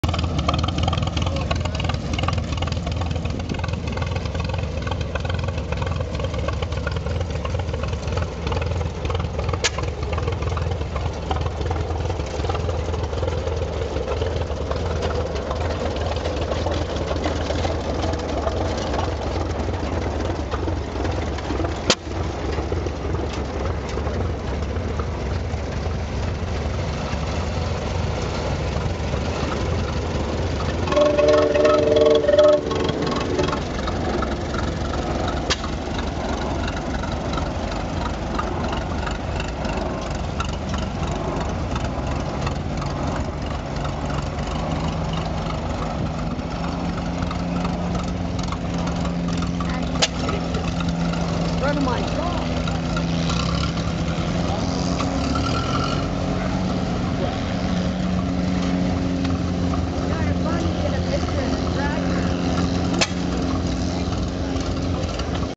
1919 International Harvester Model 8-16 Kerosene Tractor Towing a Running 9HP Economy
Mike King of the Olympic Peninsula Antique Tractor and Engine Association and his beautifully restored International, putting on a great display for club members and guests at our local show, nice work Mike!  He is followed By Dale of Trusty Tractors in Shelton with his newly acquired Garford truck.  for info on our club!